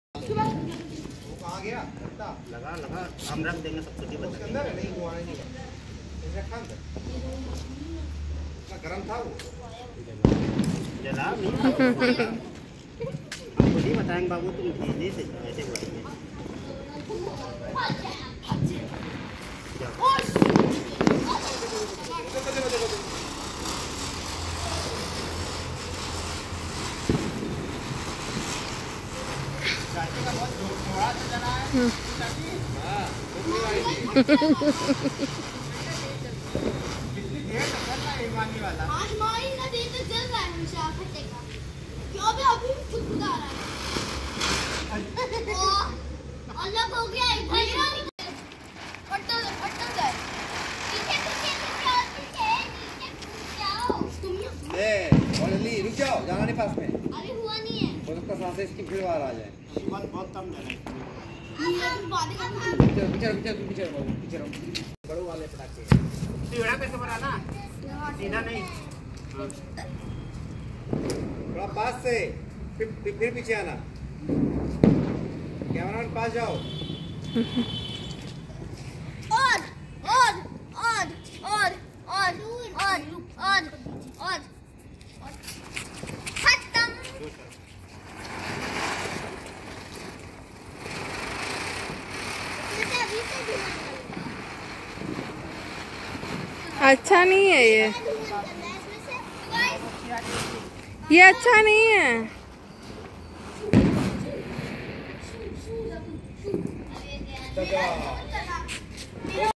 चकरघिन्नी हाथ में ही दग गई बहुत बुरा हुआ#shorts viral
चक्कर गिन्नी कभी-कभी खतरनाक हो जाती है